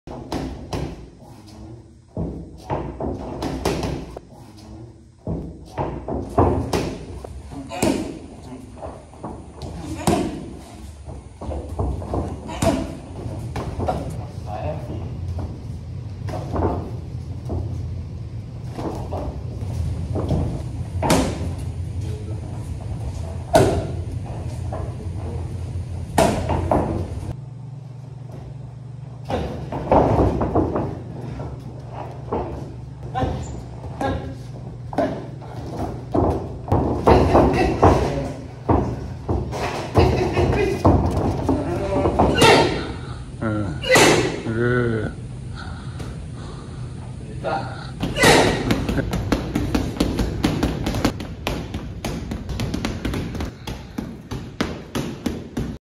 Good boxing practice with Chenet Savoi
Another productive day in the office🥊 @sparta_1804_ 👍🏽. Еще один продуктивный день в офисе 🥊👍🏽